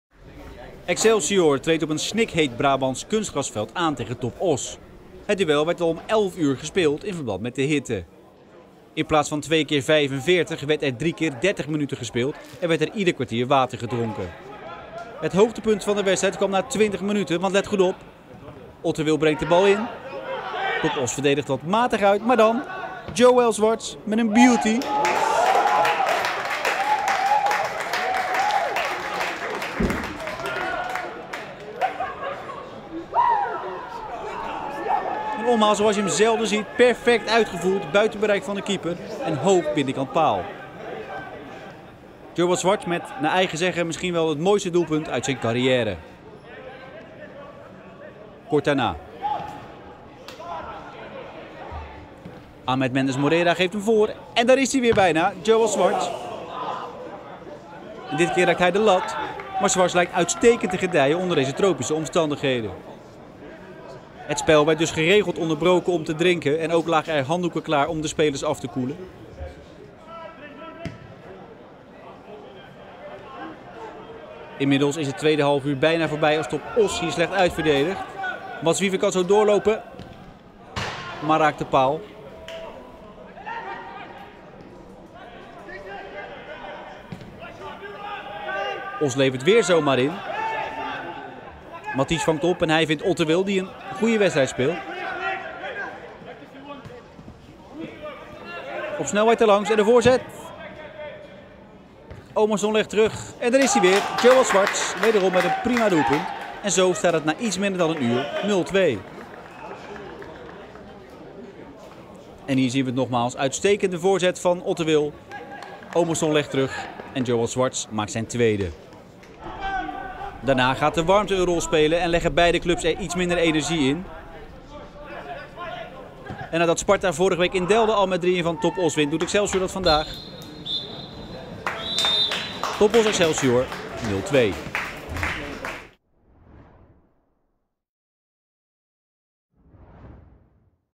Samenvatting: Fabelachtige omhaal Joël Zwarts bij oefenzege Excelsior (2-0)
Excelsior heeft zaterdag de oefenwedstrijd tegen TOP Oss met 2-0 gewonnen. Op het snikhete kunstgrasveld sprong de 0-1 uit het oog: een fantastische omhaal van Joël Zwarts.De aanvaller van Excelsior zag halverwege de eerste helft de bal aan de zijkant van het strafschopgebied, bedacht zich geen moment en scoorde met een fantastische omhaal. Later deze middag verschijnt een samenvatting van de wedstrijd, met uiteraard de beelden van de wereldgoal van Joël Zwarts. Het duel was vanwege de hitte vervroegd naar 11:00 uur. Er werd drie keer dertig minuten gevoetbald en er was ieder kwartier een drinkpauze. Aan het begin van het tweede deel van de wedstrijd gingen de sproeiers aan. De spelers voetbalden gewoon door.Ook in dit deel van het duel was Excelsior beter. Na dom balverlies van de TOP Oss-verdediging schoot Mats Wieffer op de paal. In de aanval die daarop volgde, pompte Brandon Ormonde Ottewil de bal de zestien in, kopte Omarsson de bal goed terug en schoot Zwarts van dichtbij raak: 0-2.In het laatste half uur waren de krachten bij beide ploegen wat weg en kreeg alleen TOP Oss nog een grote kans om te scoren, maar de thuisclub stuitte op doelman Alessandro Damen.